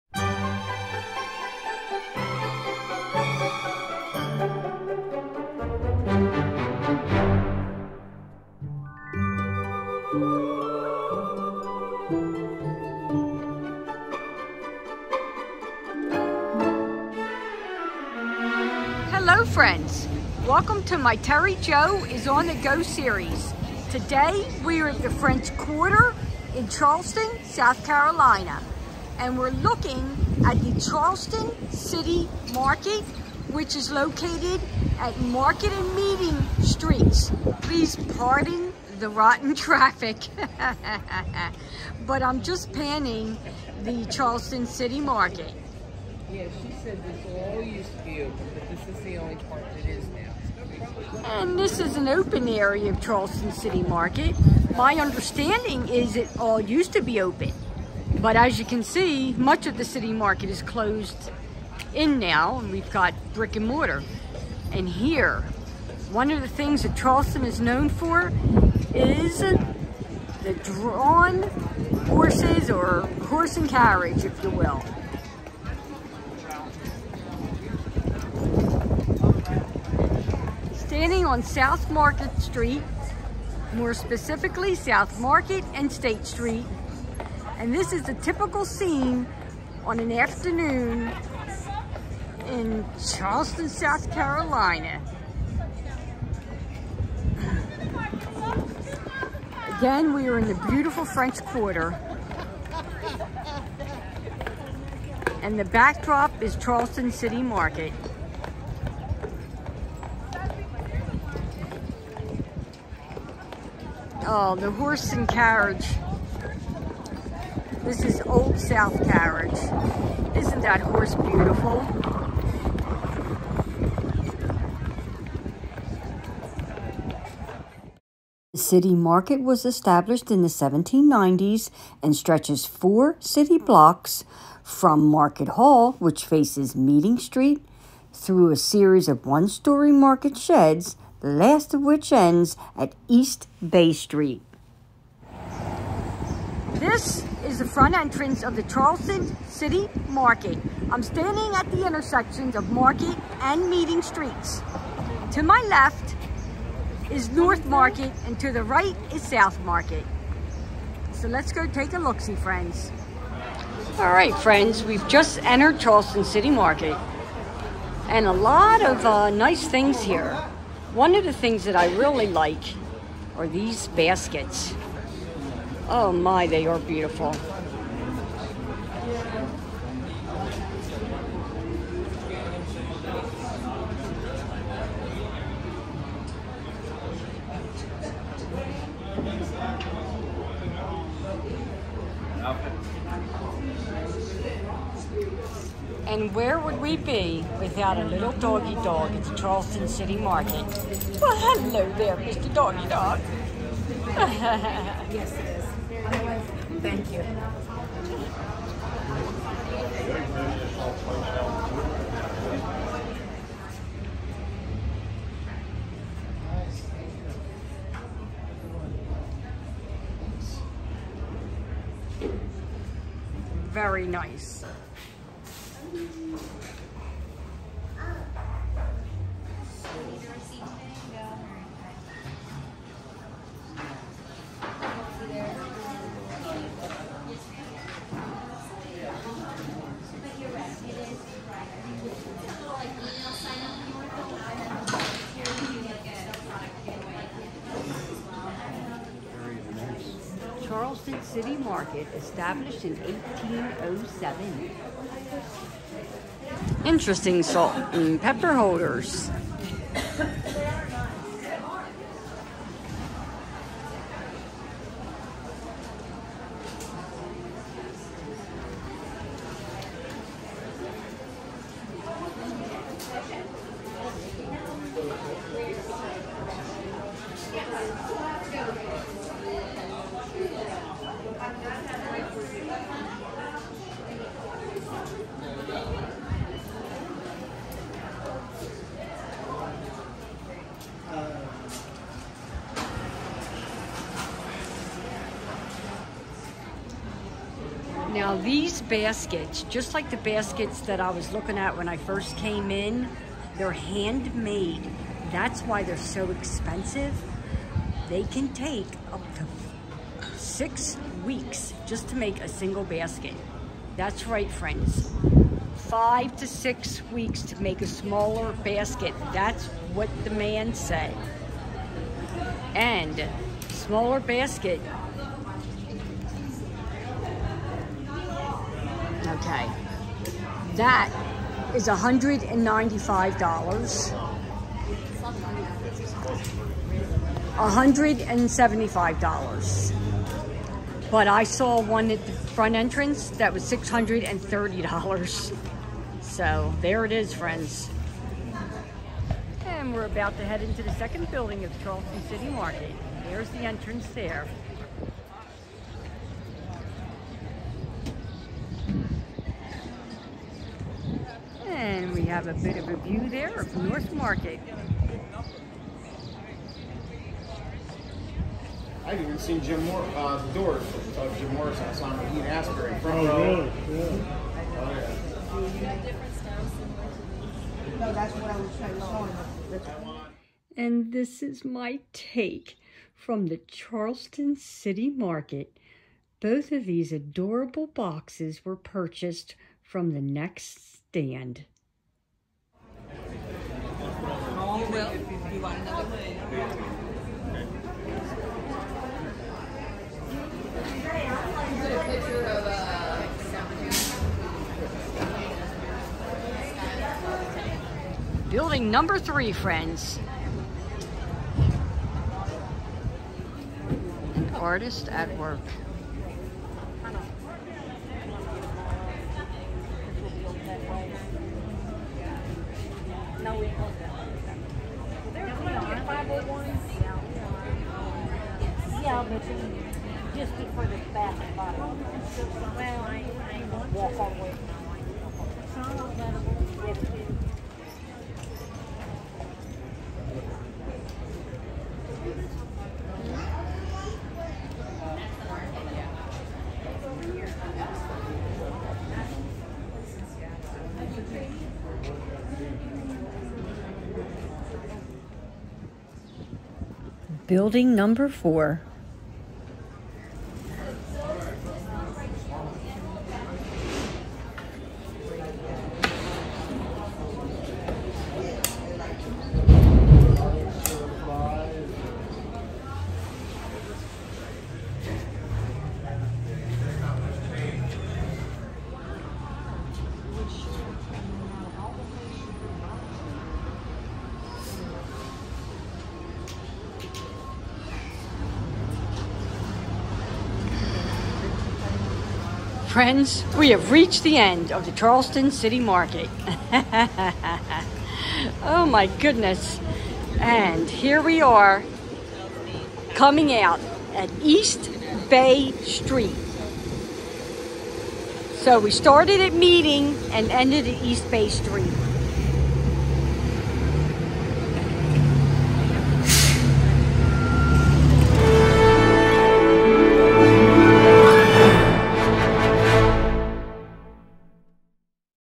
Charleston City Market
This video was filmed on April 28th, 2022 in Charleston, South Carolina. This video features the Charleston City Market. Charleston’s first public market was established in 1692 at the corner of Broad and Meeting streets. The first market sheds were built in 1790, and gradually expanded to occupy most of this strip from the Charleston Harbor to Market Street by 1806.  In 1838, a fire destroyed the market’s head-house, and the city enlisted local architect Edward B. White (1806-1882) to design the current head-house, Market Hall, which was completed in 1841.￼ Following Hurricane Hugo in 1989, the Market Hall building was restored by the City of Charleston and received a Carolopolis Award from the Preservation Society in January 2003.